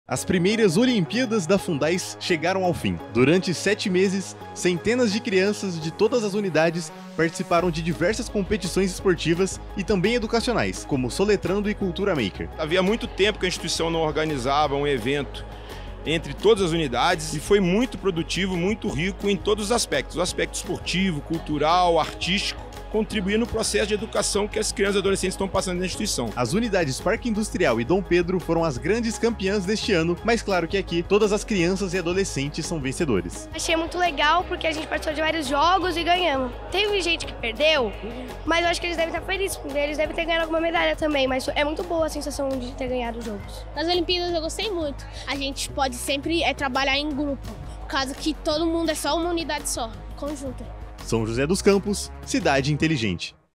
FUNDHAS ENCERRA 1ª OLIMPÍADHAS E PREMIA ALUNOS | ACONTECE SÃO JOSÉ
🏅 A Fundhas realizou neste ano a 1ª edição das Olimpíadhas! As disputas movimentaram alunos de todas as unidades, que foram premiados ao final da competição.

▶️ A equipe do Acontece foi até a cerimônia de encerramento na sede para conversar com os alunos campeões, confira!

Imagens: Luiz Big/PMSJC
Reportagem: Lucas Brito/PMSJC